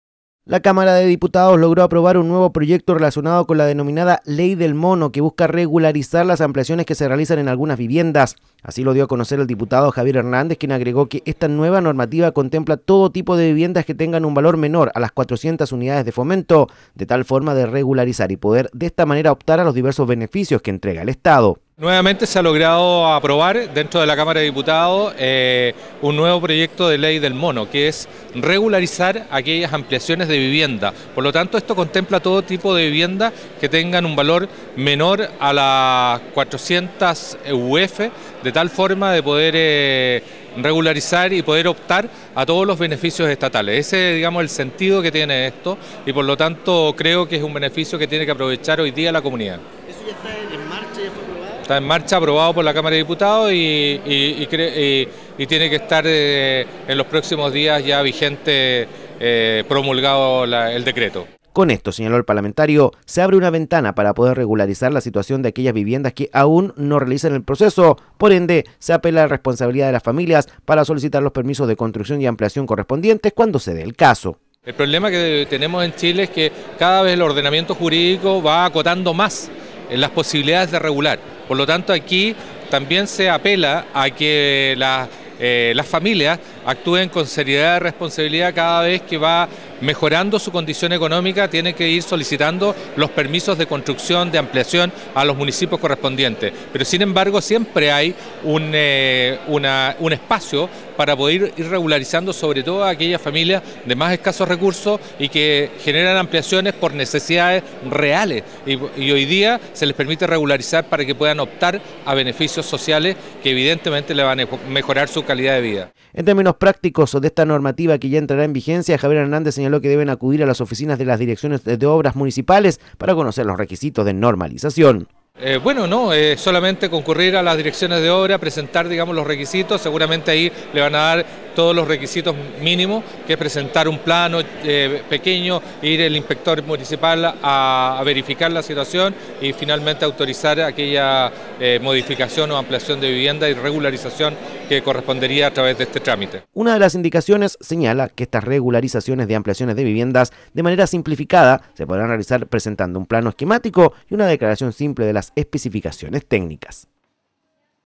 Cámara de Diputados aprueba "ley del mono"
Cámara de Diputados aprobó la denominada "ley del mono" que busca regularizar las construcciones y ampliaciones de viviendas.